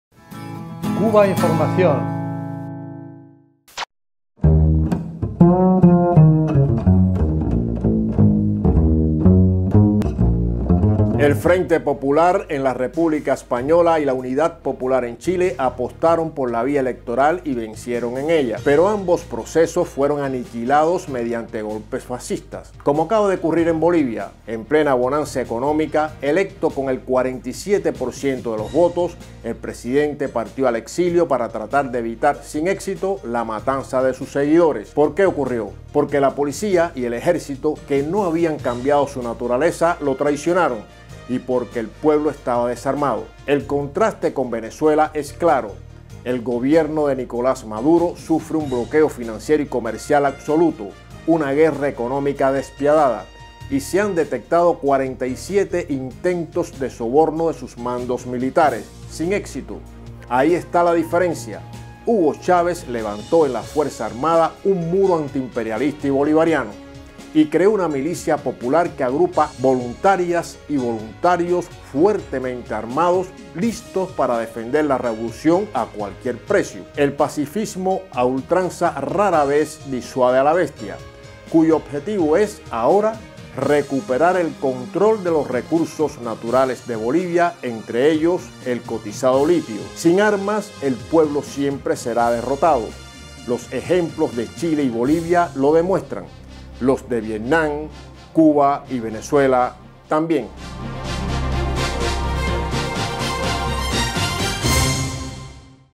Bolivia y Venezuela: pueblo desarmado, pueblo armado
Cubainformación TV – Basado en un texto de Bruno Guige – RT France.- El Frente Popular en la República española y la Unidad Popular en Chile apostaron por la vía electoral y vencieron en ella. Pero ambos procesos fueron aniquilados mediante golpes fascistas. Fuente